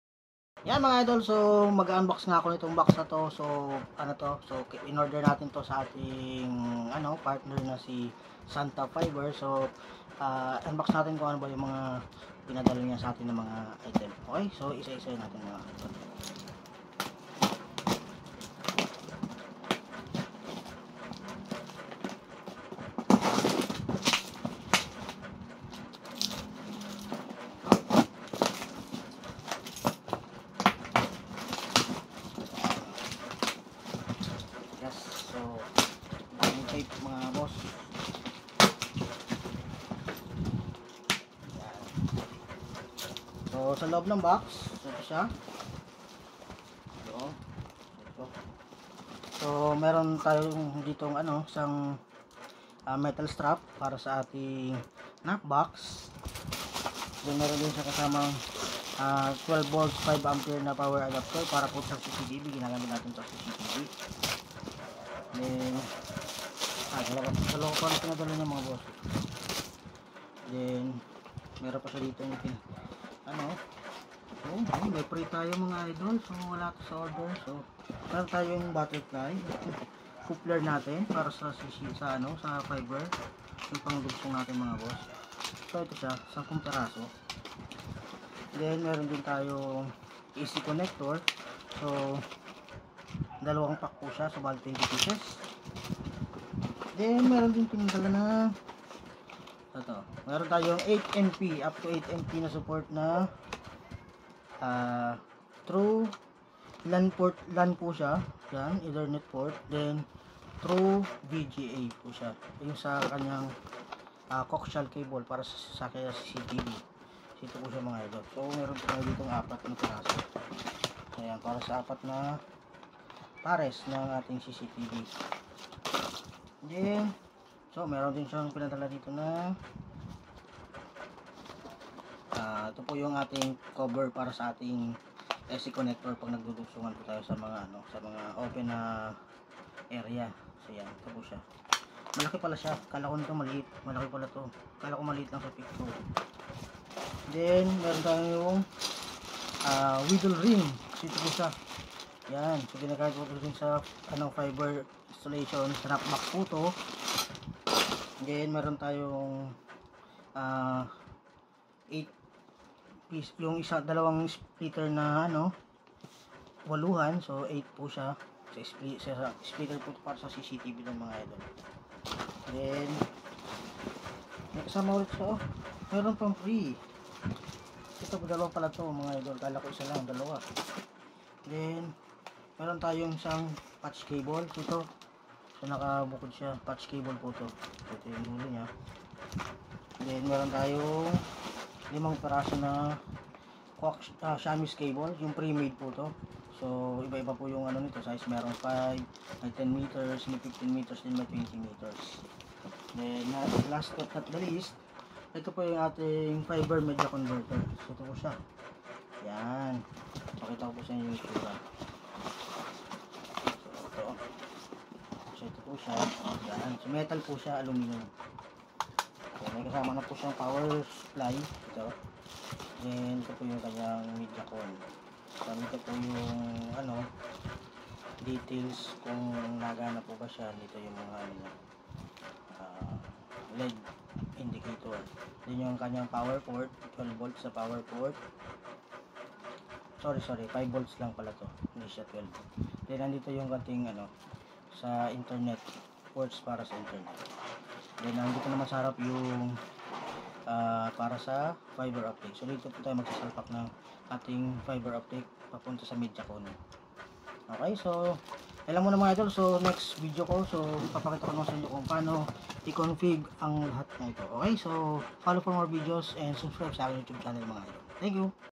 Unboxing Fiber and CCTV parts used for installation
unbox natin etong pinadala ni zantop fiber na mga parts ng cctv at fiber para sa pag iinstall